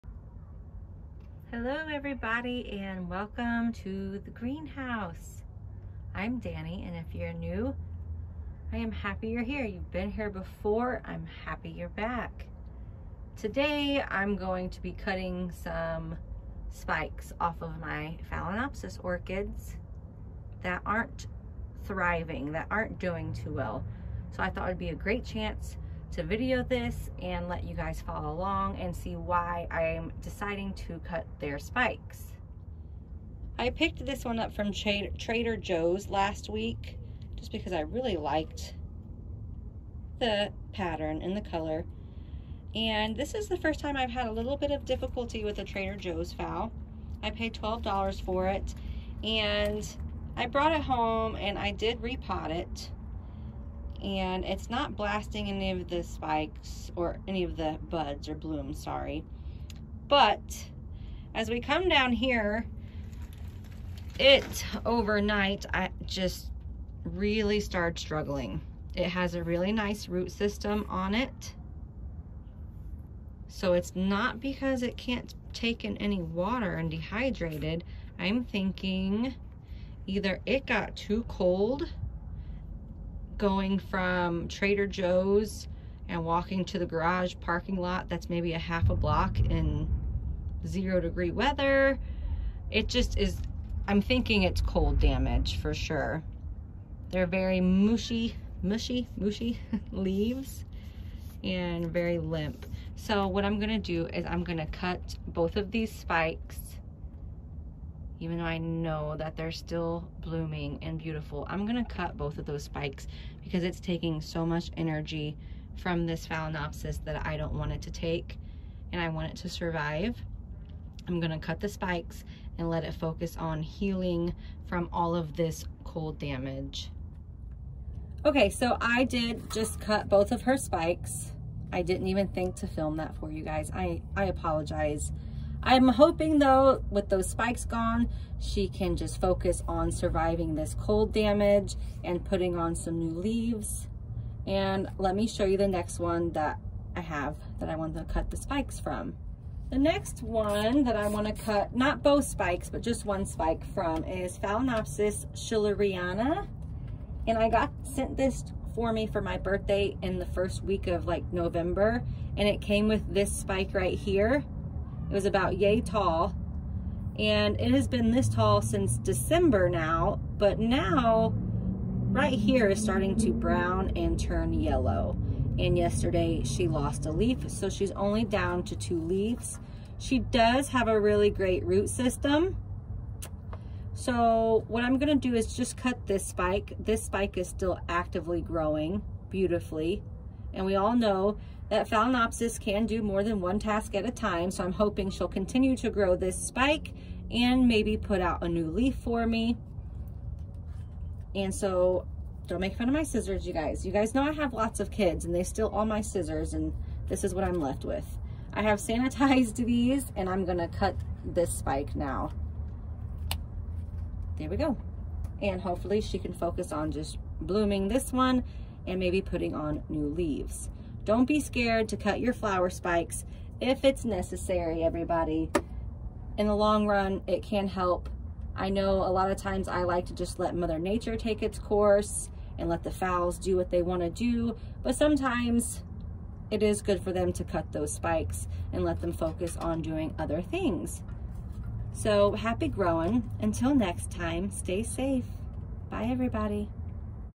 Cutting Spikes On Phalaenopsis That Aren’t Thriving. Phalaenopsis Orchid Care For Beginners.
Cutting spikes off of two Phalaenopsis. One with cold damage and one that isn’t thriving, stalling spikes, dropping leaves. Sometimes it’s best to cut those spikes and let the orchid focus on other things IF that orchid is struggling.  Happy growing. 💜